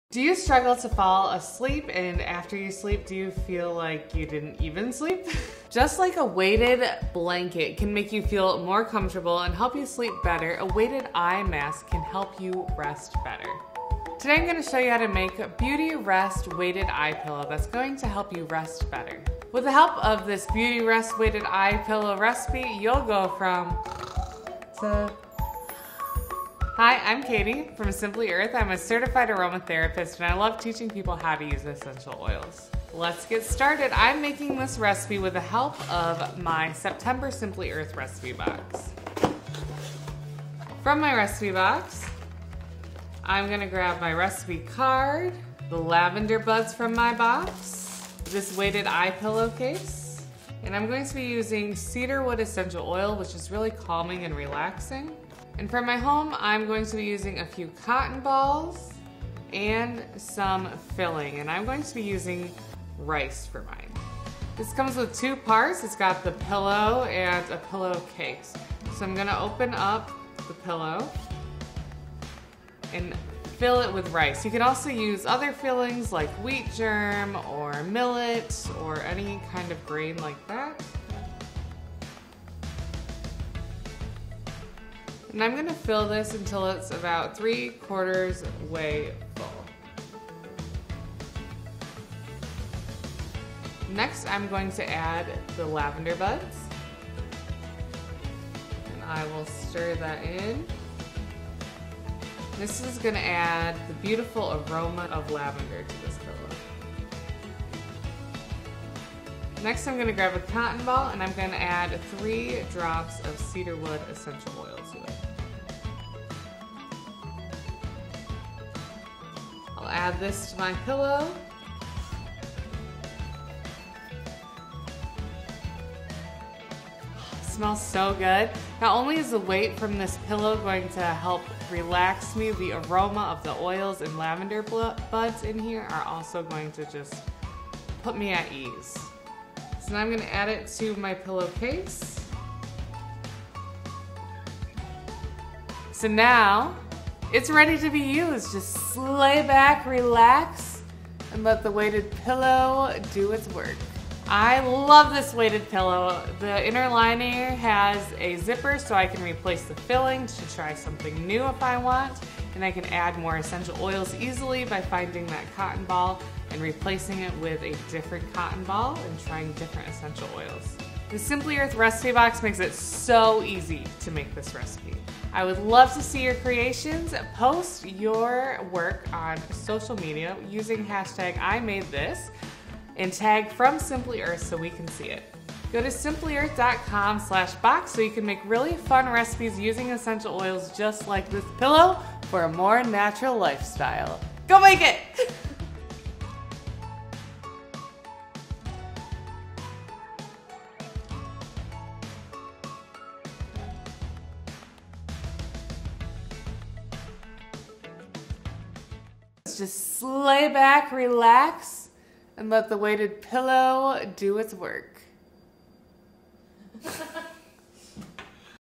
Beauty Rest Aromatherapy Weighted Eye Pillow
A good night's sleep is important for overall health and one way to get the most out of your slumber is to use an eye pillow.

Let the soothing aroma of essential oils and the gentle weight of Beauty Rest Weighted Eye Pillow help you drift off into a deep slumber. You'll wake up feeling refreshed and well-rested, I promise!

Here's what you'll learn in this video:
■ How to use this recipe